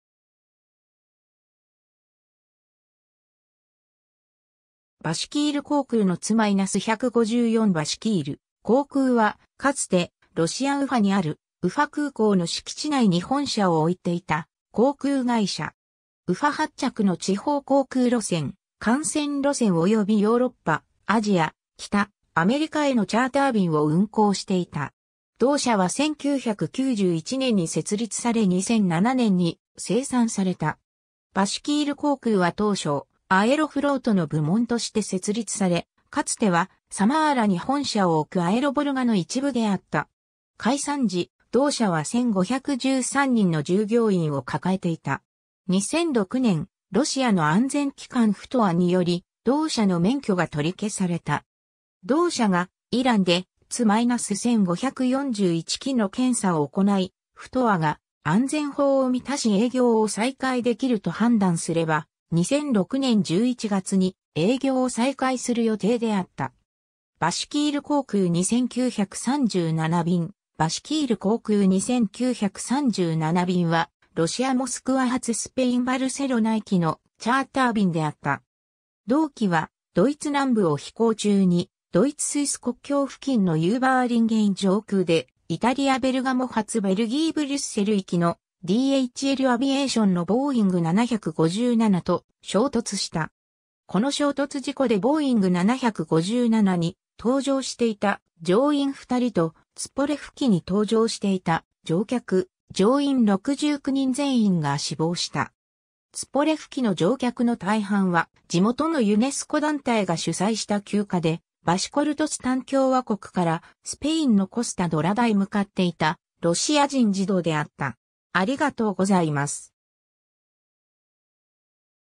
バシキール航空
バシキール航空のTu-154（2005年にモスクワで撮影） バシキール航空（バシキールこうくう、英語: BAL Bashkirian Airlines、ロシア語: «Башкирские авиалинии»）は、かつてロシア・ウファにあるウファ空港の敷地内に本社を置いていた航空会社。
ウファ発着の地方航空路線・幹線路線およびヨーロッパ、アジア、北アメリカへのチャーター便を運航していた。
同社は1991年に設立され2007年に清算された。
 バシキール航空は当初アエロフロートの部門として設立され、かつてはサマーラに本社を置くアエロヴォルガの一部であった。
解散時、同社は1,513人の従業員を抱えていた。
2006年、ロシアの安全機関FTOAにより同社の免許が取り消された。
同社がイランでTu-1541機の検査を行いFTOAが安全法を満たし営業を再開できると判断すれば、2006年11月に営業を再開する予定であった。
 バシキール航空2937便（2002年）: バシキール航空2937便 (RA-85816) はロシア・モスクワ発スペイン・バルセロナ行きのチャーター便であった。
同機 (Tu-154) はドイツ南部を飛行中に、ドイツ・スイス国境付近のユーバーリンゲン上空でイタリア・ベルガモ発ベルギー・ブリュッセル行きのDHLアビエーションのボーイング757と衝突した。
この衝突事故でボーイング757に搭乗していた乗員2人とツポレフ機に搭乗していた乗客・乗員69人全員が死亡した。
ツポレフ機の乗客の大半は、地元のUNESCO団体が主催した休暇でバシコルトスタン共和国からスペインのコスタ・ドラダ (Costa Daurada region) へ向かっていたロシア人児童であった。